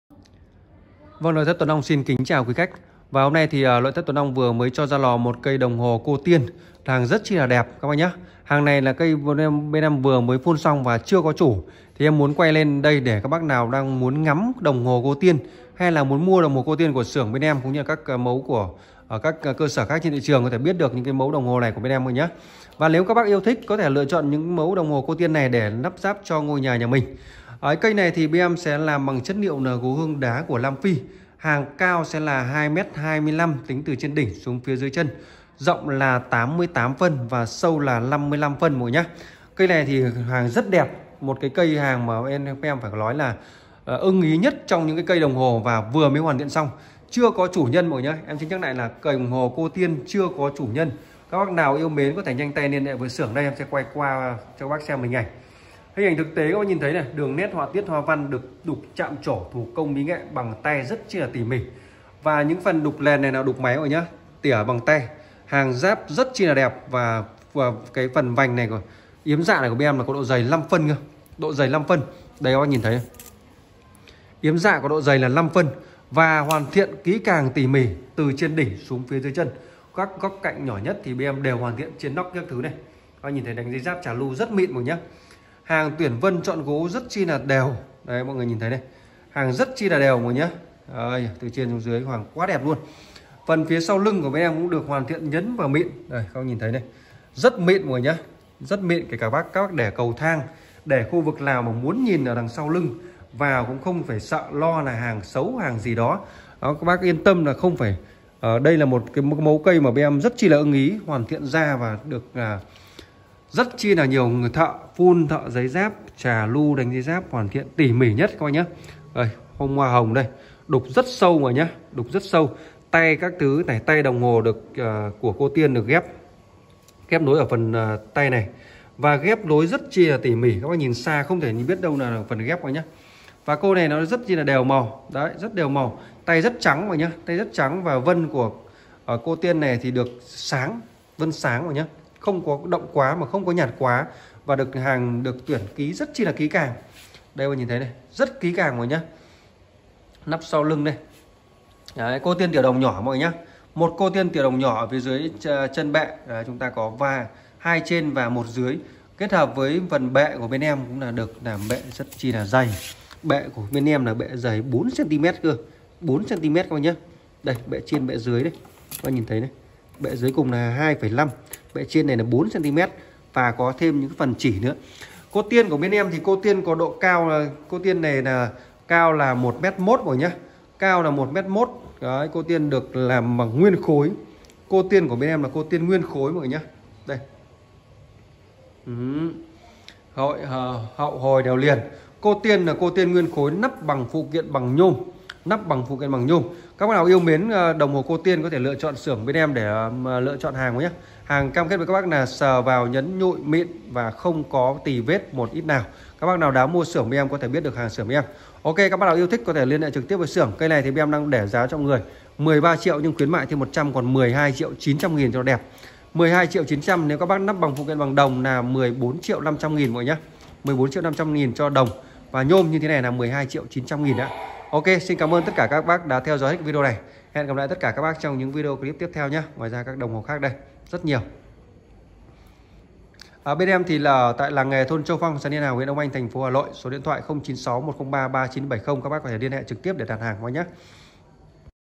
Cô tiên ❤️chưa có chủ | Đồng hồ Cây gỗ hương cao 2m25 Vân nuột không tỳ vết tại nội thất Tuấn Long
Xin chào các Bác, nay e có mẫu Cô tiên ❤️chưa có chủ | Đồng hồ Cây gỗ hương cao 2m25 Vân nuột không tỳ vết tại nội thất Tuấn Long

- Đồ gỗ nội thất Tuấn Long: Làng nghề thôn Châu Phong - xã Liên Hà - huyện Đông Anh - Tp Hà Nội